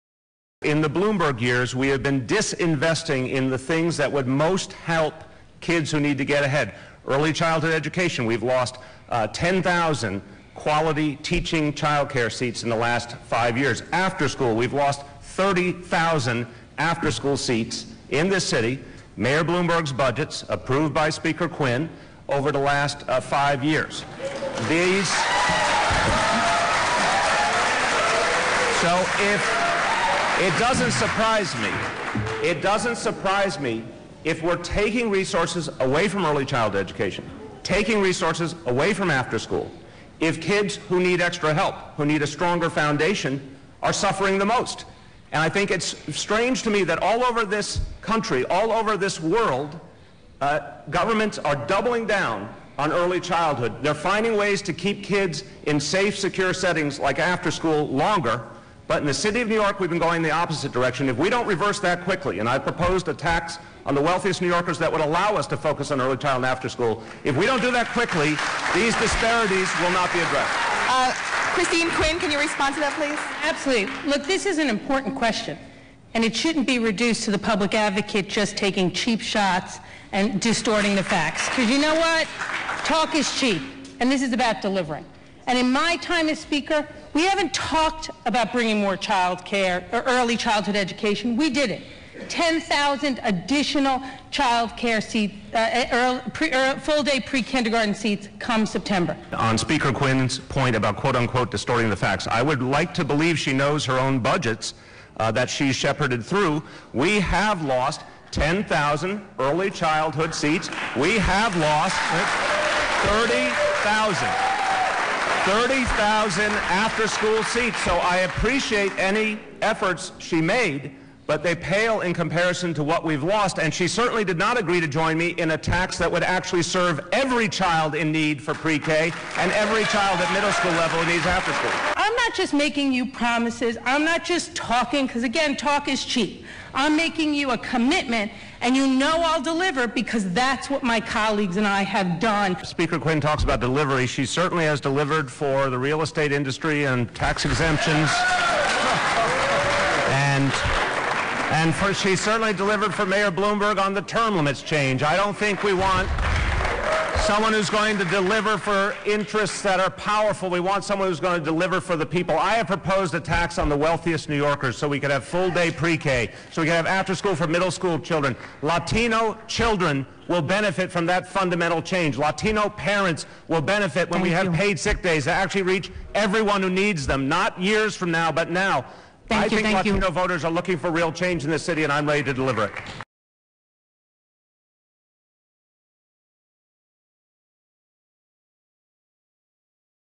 Bill de Blasio Dominates NY1Debate
Bill de Blasio emerged as the clear progressive choice at the second televised debate on June 19th. Take a look at some key moments from the debate, and then say you stand with Bill.